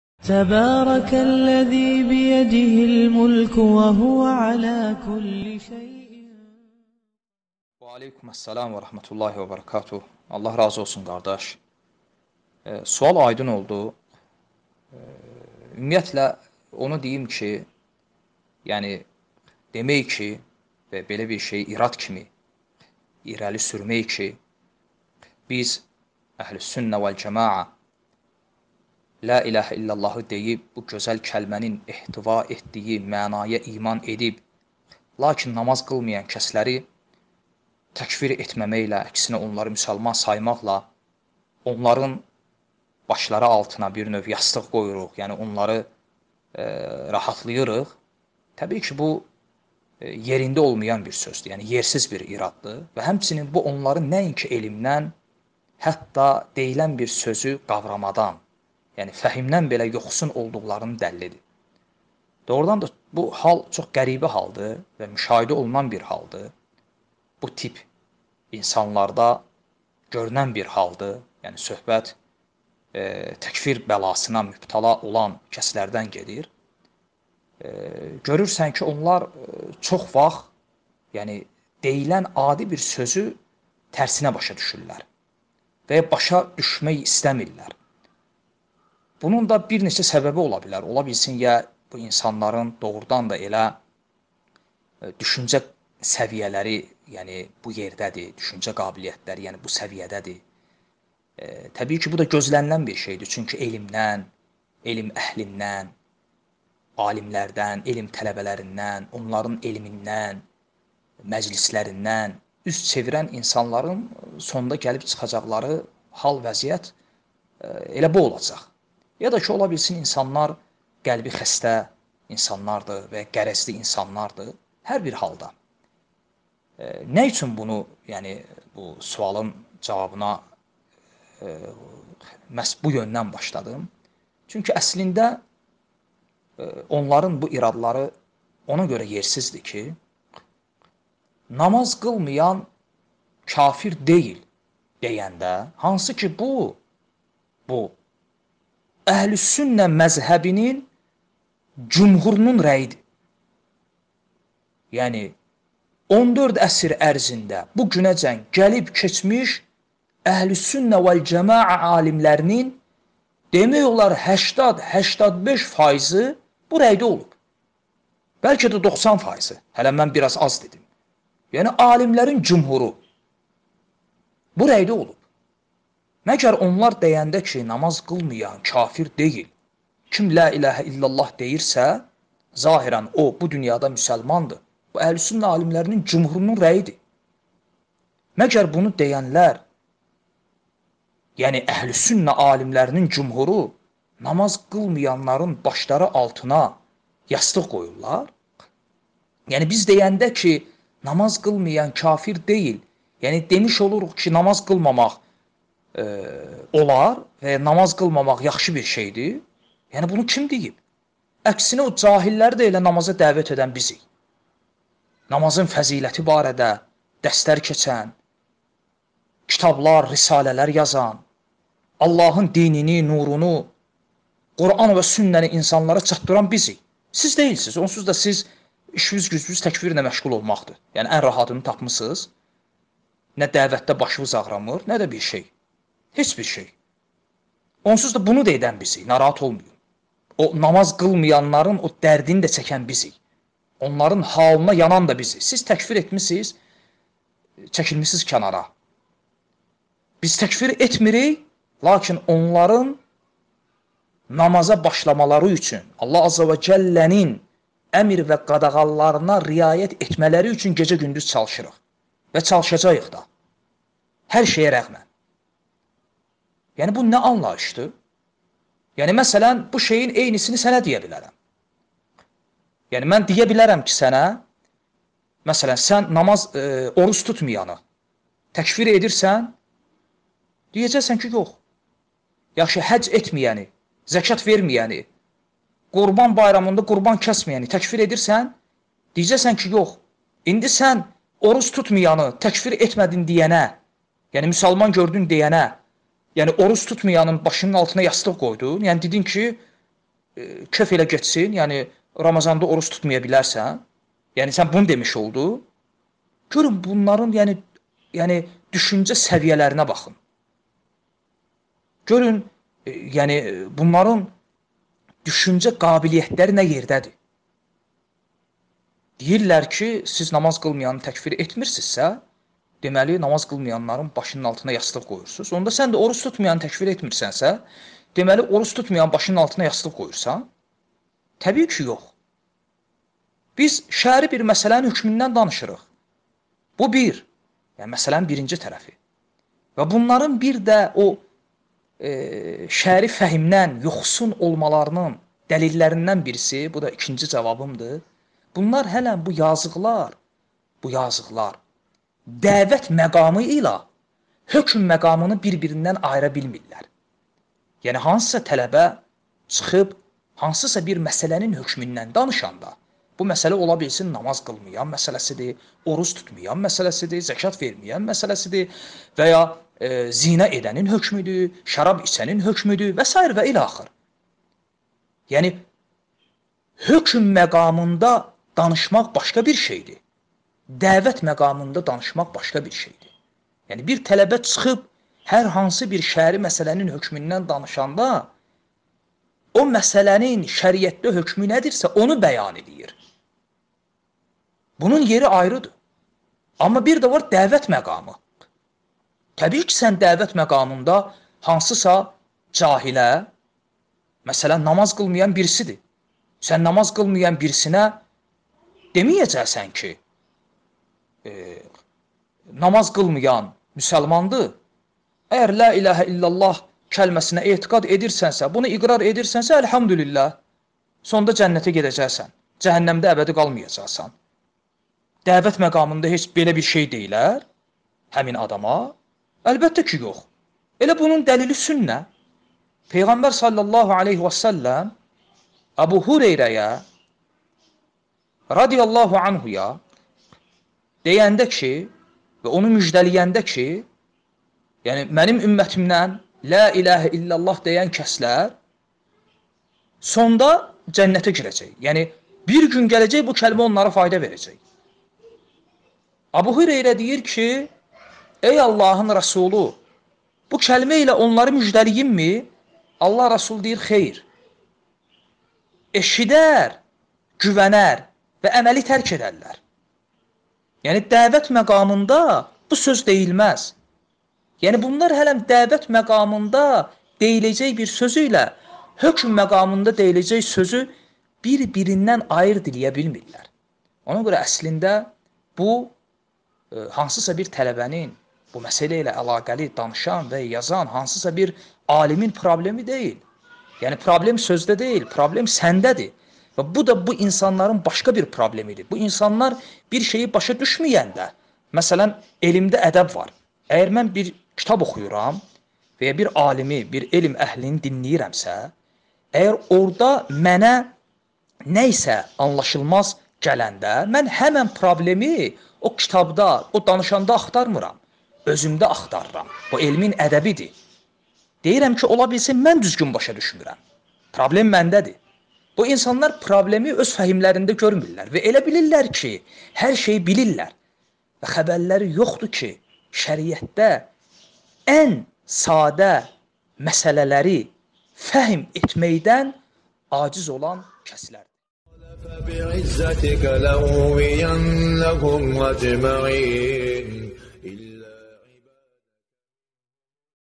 Namaz qılmayanı müsəlman görməklə onların başları altına yastıq qoyursuz ⛔şübhəsinə cavab(20.05.21)
Namaz qılmayanı müsəlman görməklə onların başları altına yastıq qoyursuz ⛔şübhəsinə cavab(20.05.21) Abu Zeyd